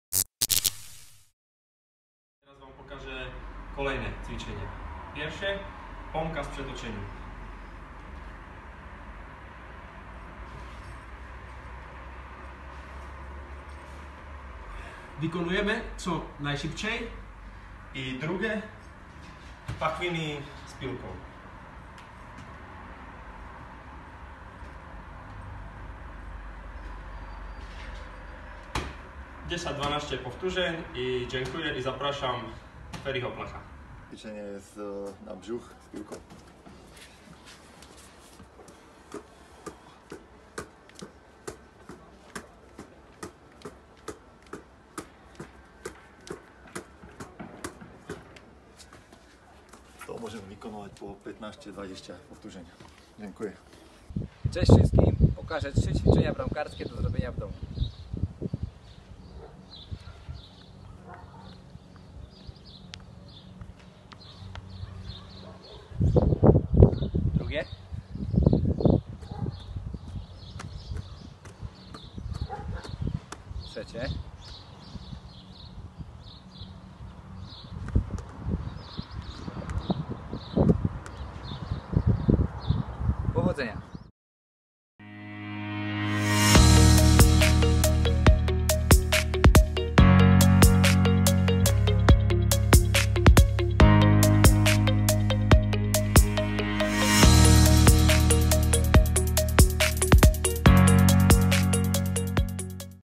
TRENING | Ćwicz z Piastem!
W kolejnym odcinku wspólnych ćwiczeń w domu rolę trenerów pełnią: Patryk Królczyk, Tomas Huk oraz Fero Plach ! Zostańwdomu i zadbaj o swoją kondycję oraz samopoczucie w mistrzowski sposób!💪🏽😄